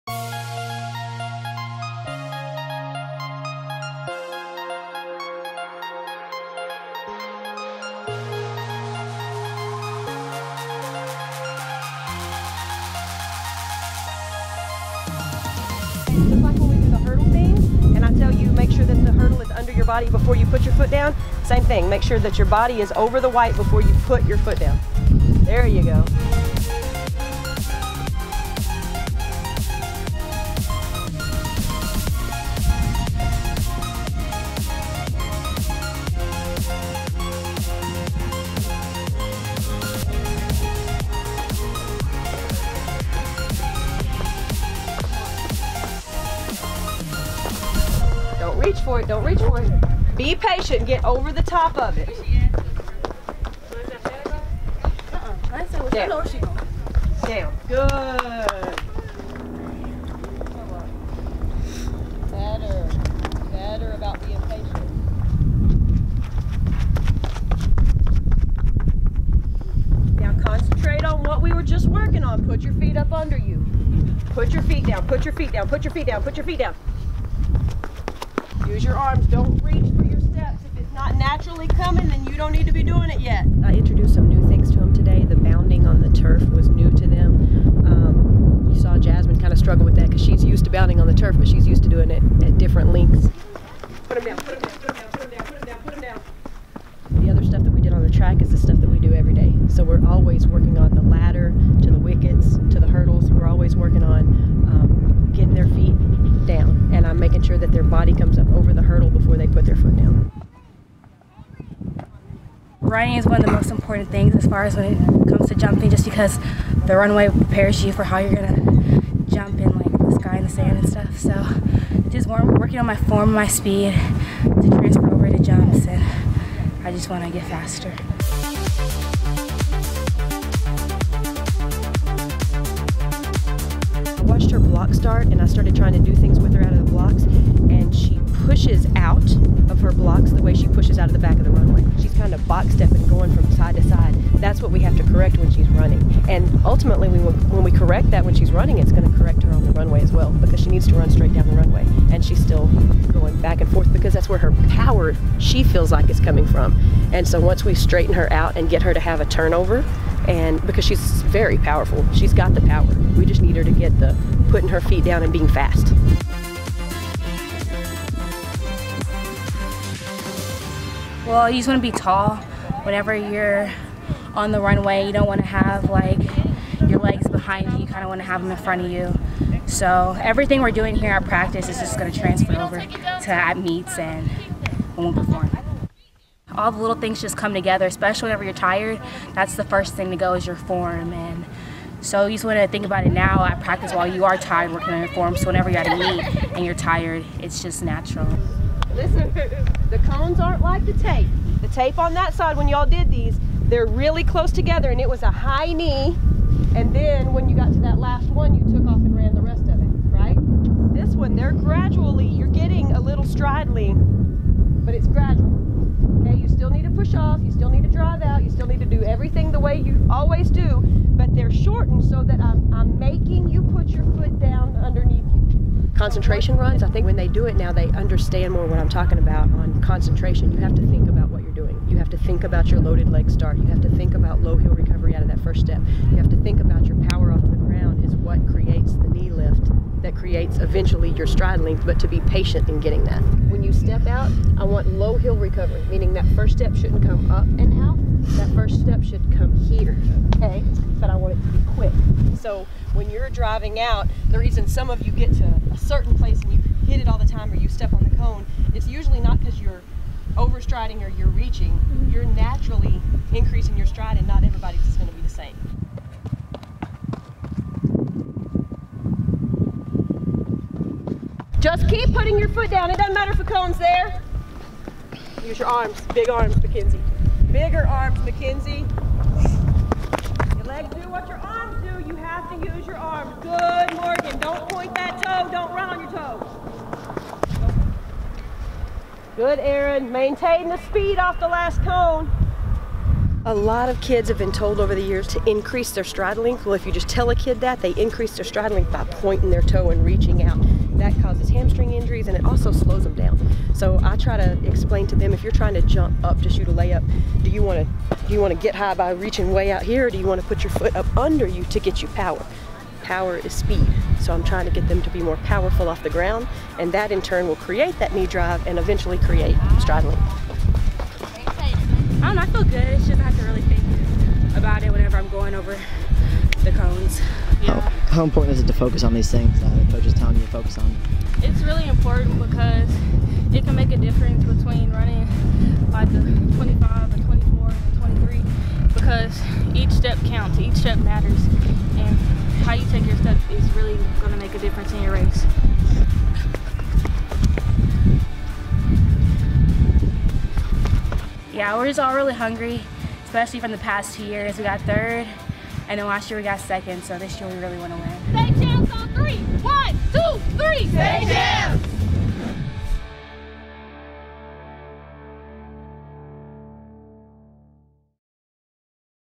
Workout Wednesday: Jasmine Moore Triple Jump Practice
Jasmine Moore is one of the best triple jumpers of this decade. She ranks as the seventh-best triple jumper all-time and has the 13th-best jump all-time with her jump of 42 feet 9.5 inches. She wants to be known as the best high school jumper in history by the time she graduates.